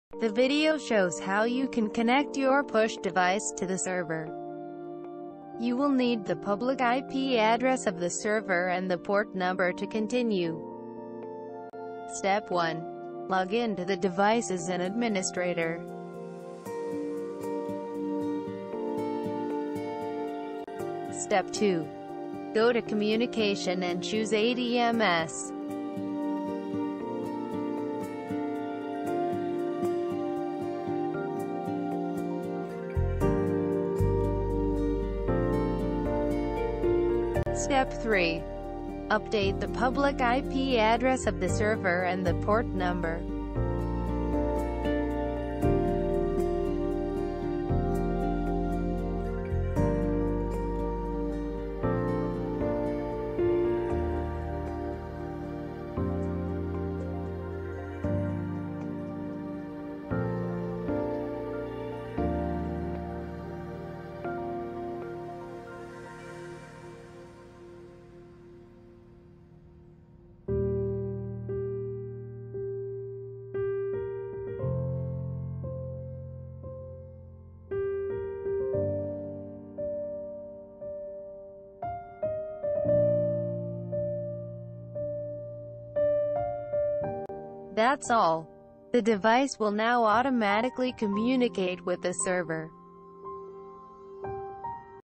How to connect your PUSH device(ZKTeco Uface 202) to the server.
The video shows how you can connect your PUSH device ZkTeco Uface 202 to the server.